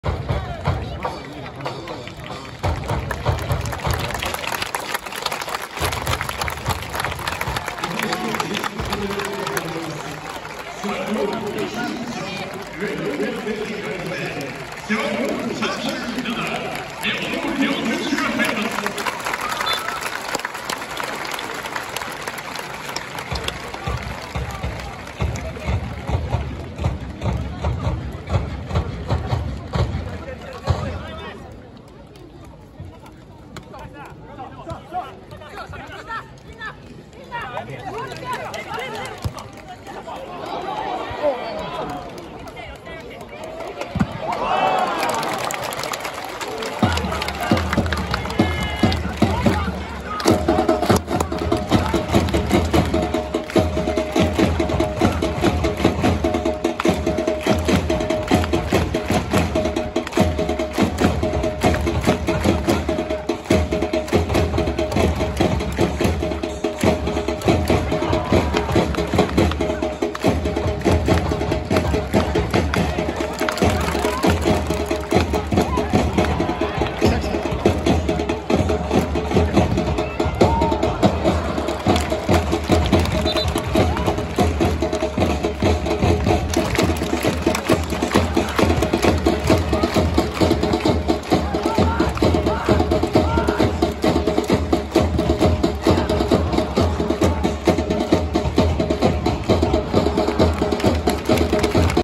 新加入 根本凌選手投手から
栃木SC vs大分トリニータ　根本凌選手投入からの動画です！